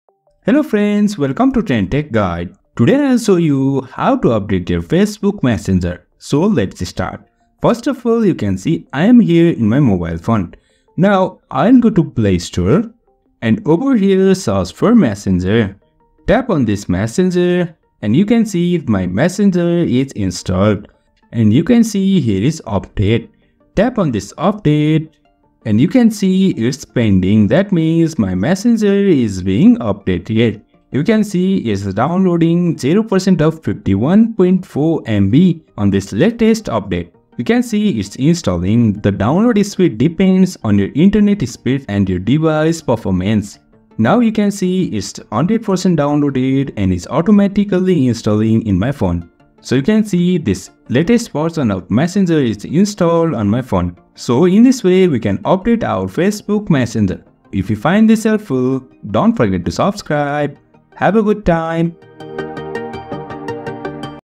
How to Update Facebook Messenger on Android | Easy Tutorial
This video talks about how to update Facebook Messenger on Android. Stay up-to-date with the latest features and improvements on Facebook Messenger by learning to update the app on your Android device. Keep your messaging experience smooth and secure!

👉 Explore more!